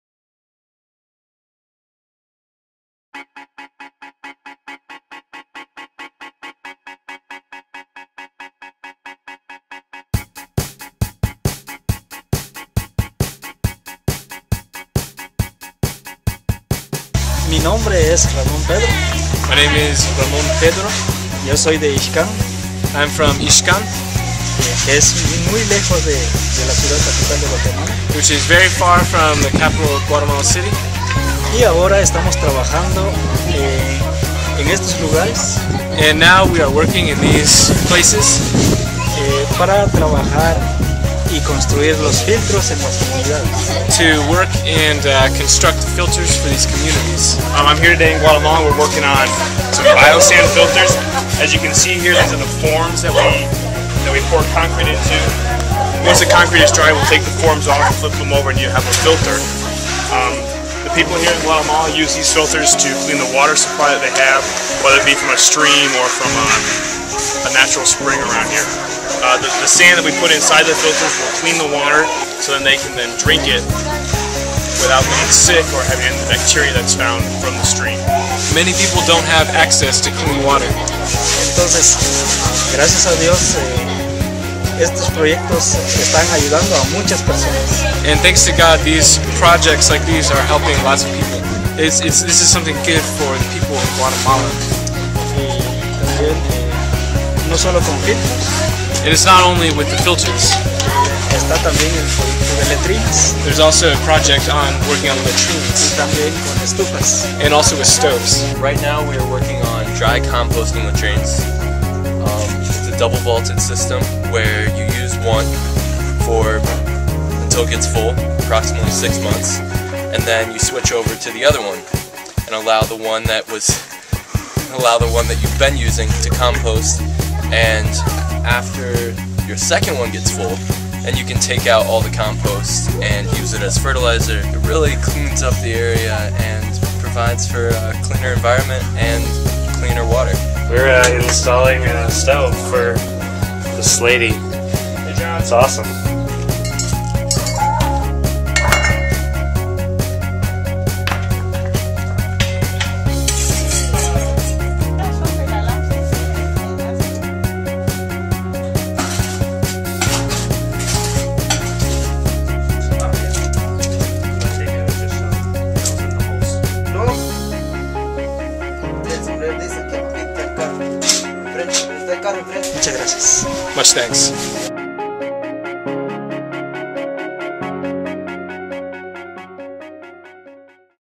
Guatemala - Taylor University - Earth & Environmental Science
Watch the Guatemala team, led by Dr. Michael Guebert, Earth and Environmental Science Department Chair and Professor, as they assemble fuel-efficient stoves, home water purifiers, composting latrines and rainwater cisterns for the Guatemalan people. Hear from one of the local Guatemalan people how these efforts result in better health and sanitation for their community.